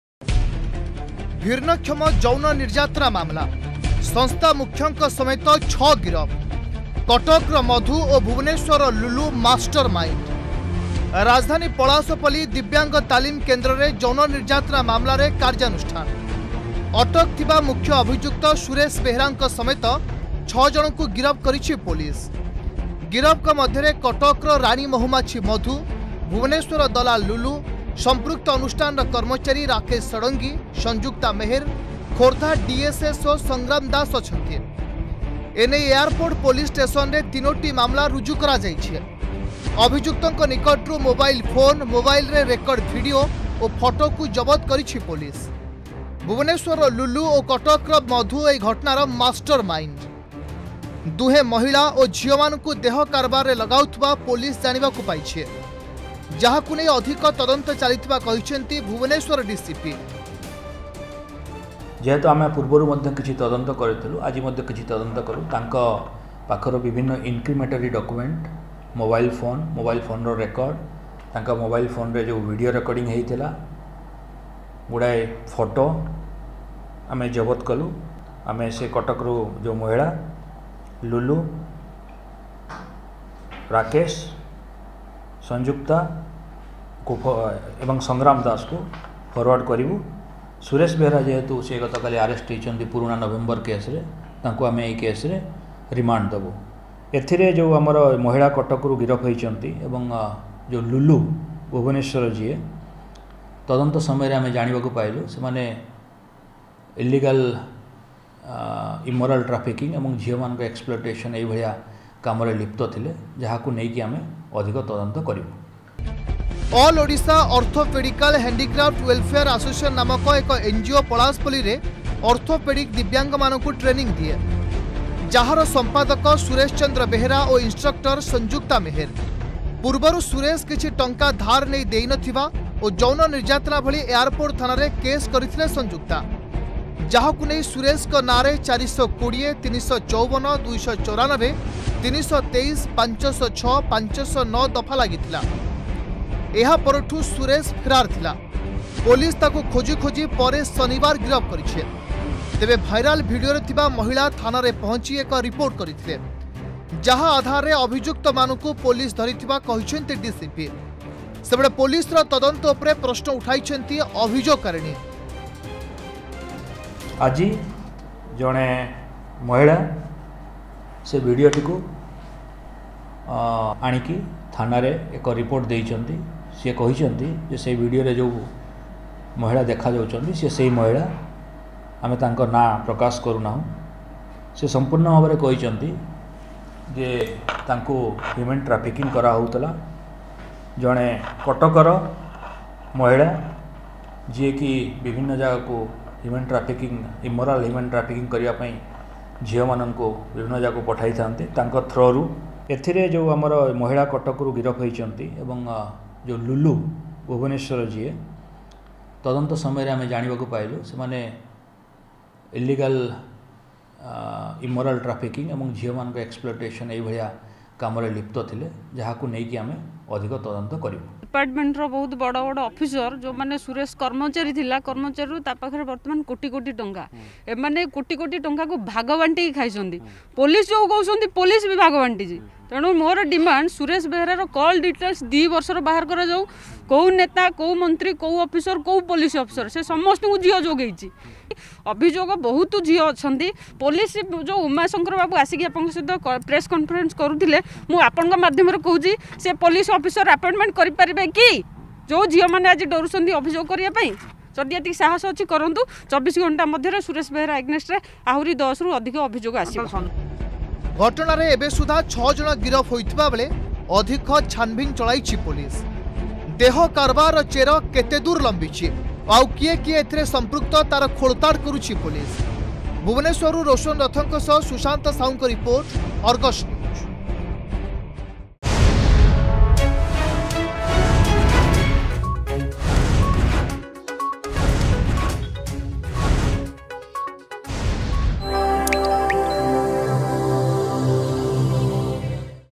ସେକ୍ସ ରାକେଟରେ ସଂସ୍ଥା ମୁଖ୍ୟଙ୍କ ସମେତ 6 ଗିରଫ
ରାଜଧାନୀ ପଲାଶପଲ୍ଲୀ ଦିବ୍ୟାଙ୍ଗ ତାଲିମ କେନ୍ଦ୍ରରେ ଯୌନ ନିର୍ଯାତନା ମାମଲାରେ କାର୍ଯ୍ୟାନୁଷ୍ଠାନ । ଅଟକ ଥିବା ମୁଖ୍ୟ ଅଭିଯୁକ୍ତ ସୁରେଶ ବେହେରାଙ୍କ ସମେତ 6 ଜଣଙ୍କୁ ଗିରଫ କରିଛି ପୋଲିସ । ଗିରଫଙ୍କ ମଧ୍ୟରେ କଟକର ରାଣୀ ମହୁମାଛି ମଧୁ, ଭୁବନେଶ୍ୱରର ଦଲାଲ ଲୁଲୁ, ସଂପୃକ୍ତ ଅନୁଷ୍ଠାନର କର୍ମଚାରୀ ରାକେଶ ଷଡଙ୍ଗୀ, ସଞ୍ଜୁକ୍ତା ମେହେର, ଖୋର୍ଦ୍ଧା ଡିଏସଏସଓ ସଂଗ୍ରାମ ଦାସ ଅଛନ୍ତି...